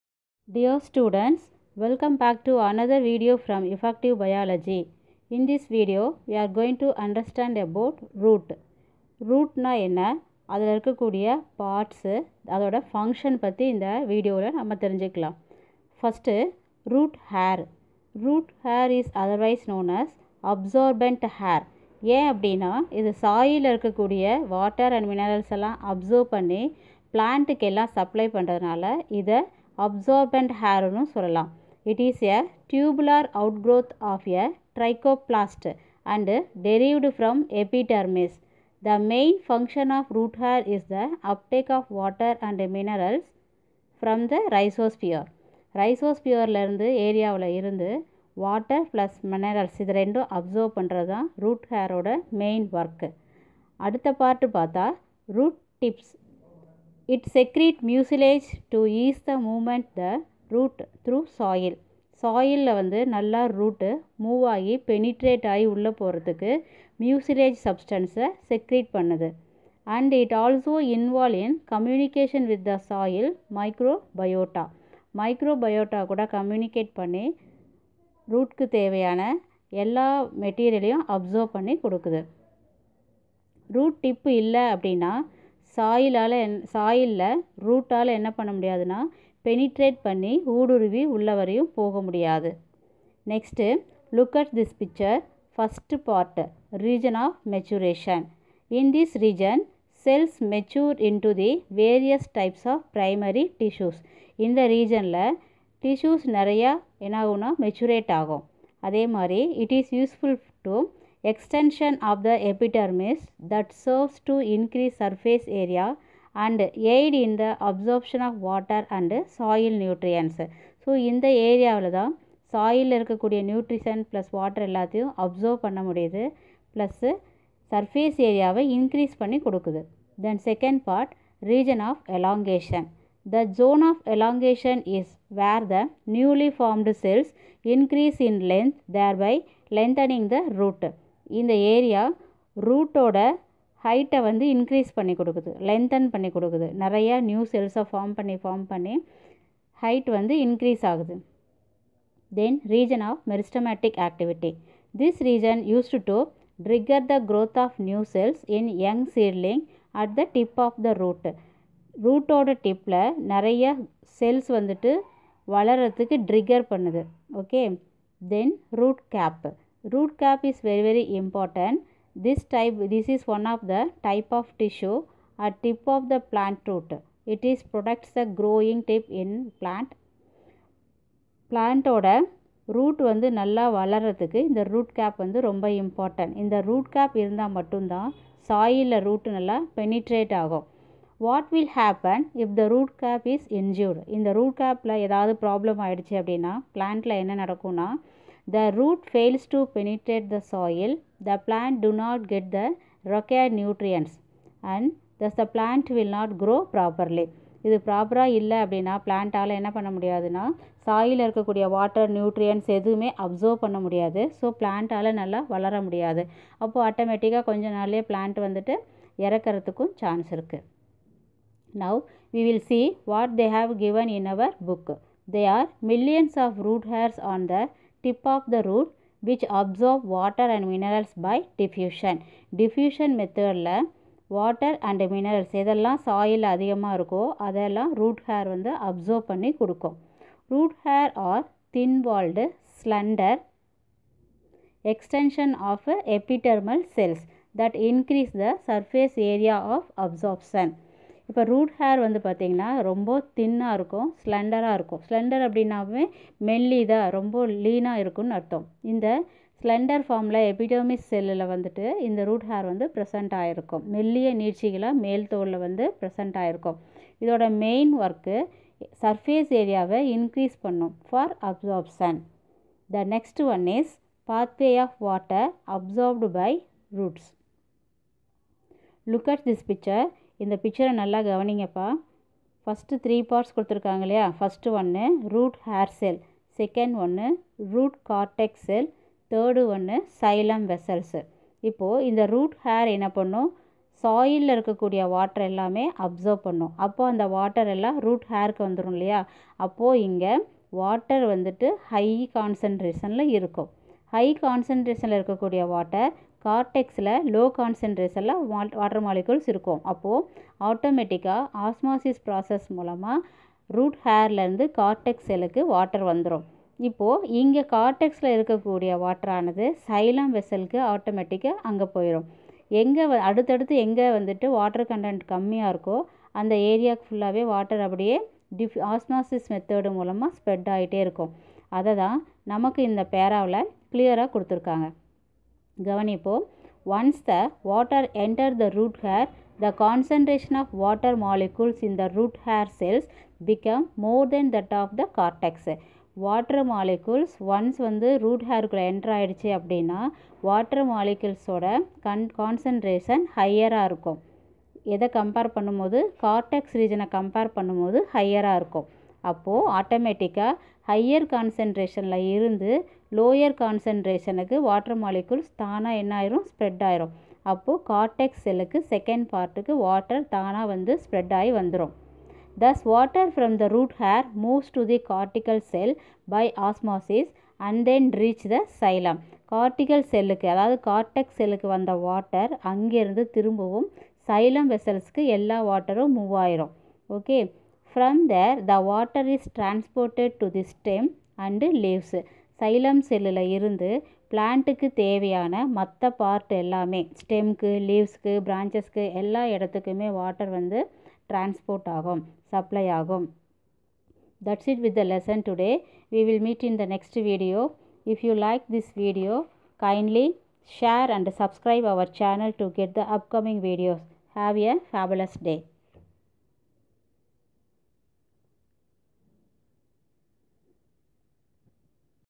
10th - Science - \\ Unit 14 \\ Root Hair - Water absorbing Unit
10th  - Science -  || Unit 14 || Transportation in Plants and Circulation in Animals - Root Hairs - Water absorbing Unit